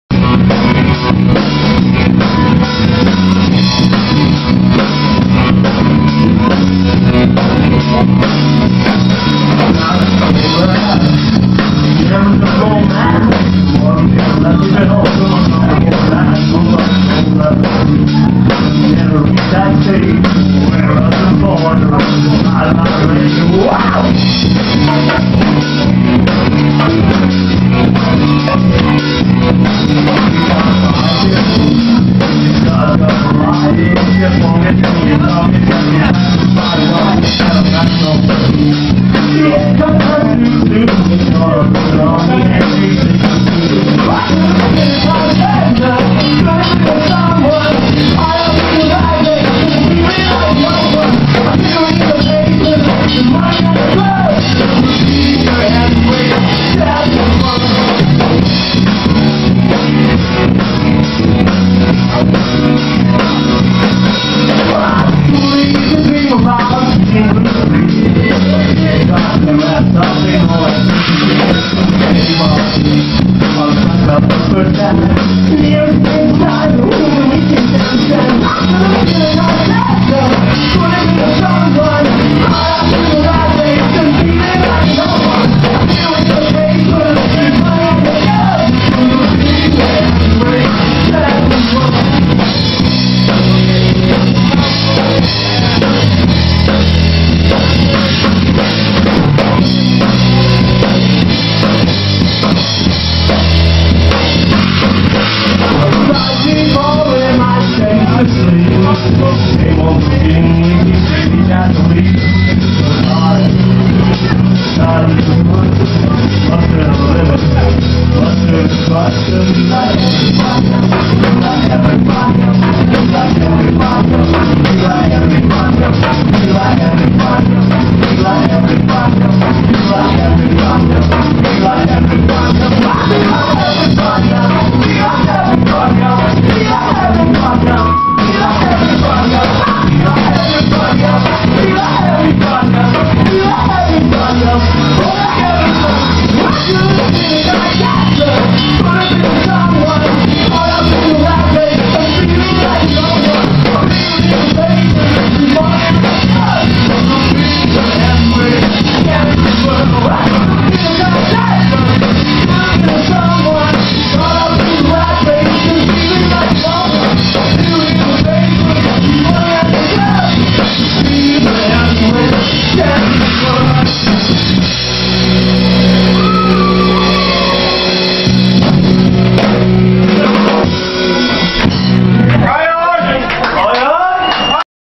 go kart commandos
Gary Dunn drumming heavyweight champion of the world in the Halfway House, Connahs Quay on Sat 31st October 2009